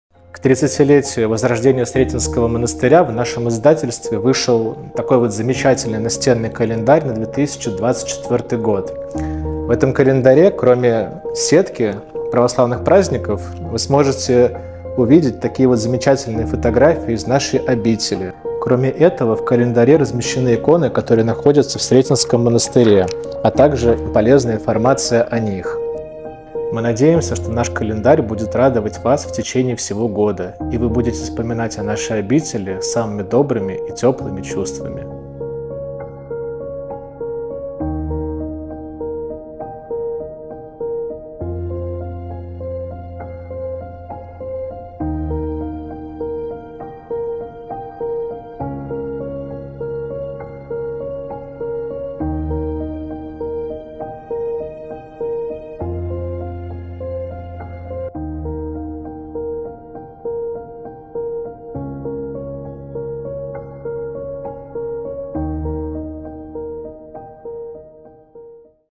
Сретенский монастырь. Календарь на 2024 год
В наступающем 2024 году исполняется 30 лет со дня возрождения Сретенского монастыря.
Более шести веков назад москвичи, благодарные Пресвятой Богородице за спасение города от войск Тамерлана, основали на месте встречи (по-славянски – сретения) Ее чудотворной Владимирской иконы Сретенский монастырь. И почти сразу он стал одним из духовных центров древней столицы. За прошедшие столетия обитель вместе со страной не раз переживала трудные времена, но самым тяжелым испытанием стало закрытие и разорение монастыря в ХХ веке.

В 1994 году началось возрождение обители. Со временем здесь появилось одно из крупнейших в России издательств, духовная семинария (ныне академия) и Духовно-просветительский центр имени священномученика Илариона (Троицкого), а также храм Воскресения Христова и Новомучеников и исповедников Церкви Русской, посвященный памяти тех, кто в годы лютых гонений бесстрашно свидетельствовал о Христе.
И вот уже 30 лет, как Сретенский монастырь снова является одним из важнейших духовных центров не только Москвы, но и всей России. Множество людей получает здесь молитвенную помощь и духовное утешение.

В издательстве Сретенского монастыря к этой знаменательной дате вышел настенный православный календарь. На его страницах содержится краткая история Сретенского монастыря, его храмов и святынь. На каждом развороте помещены замечательные тематические фотографии монастырских храмов и икон, отмечены посты, праздники, дни памяти наиболее почитаемых святых и дни особого поминовения усопших.

Настенный календарь на 2024 год, посвященный 30-летию возрождения Сретенского монастыря, станет замечательным подарком не только для его прихожан, но и для всех православных христиан, почитающих Владимирскую икону Божией Матери и любящих эту древнюю московскую обитель.

Календарь можно приобрести в магазине «Сретение» (Москва, ул. Большая Лубянка, д. 17 стр. 1) или в нашем интернет-магазине